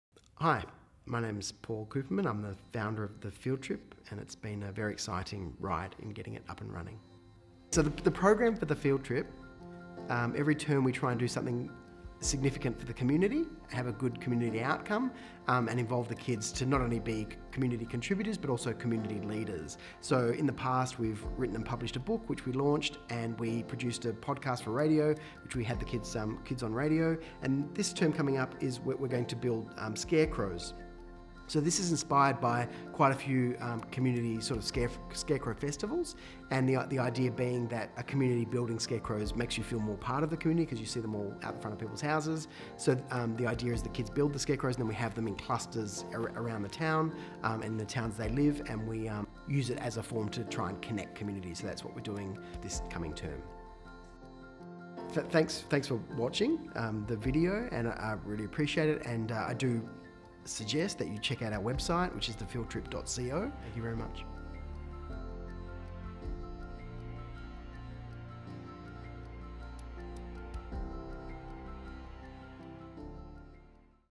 We're building scarecrows!
Field Trip Director, Paul Kooperman, talks about the program for Term 4, 2016! The Field Trip is for kids aged 10-17, sign up via the